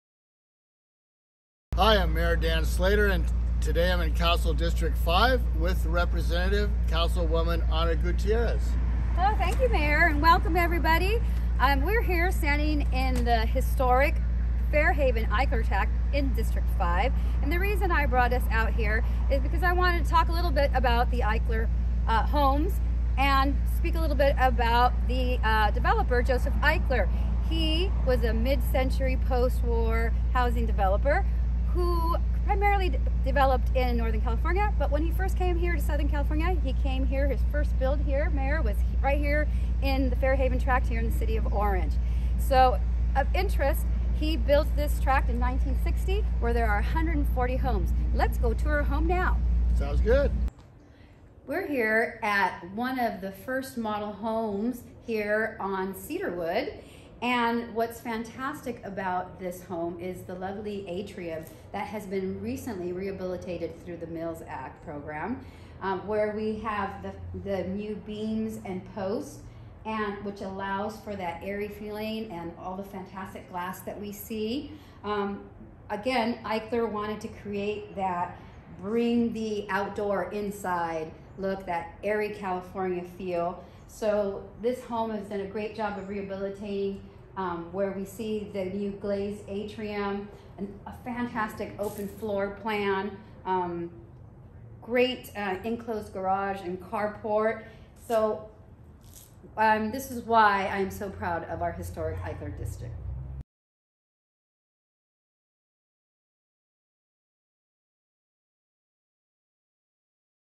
City of Orange - Minute with the Mayor (February 2024)
Mayor Dan Slater and Councilmember Ana Gutierrez take a look at Orange's historic Eichler homes.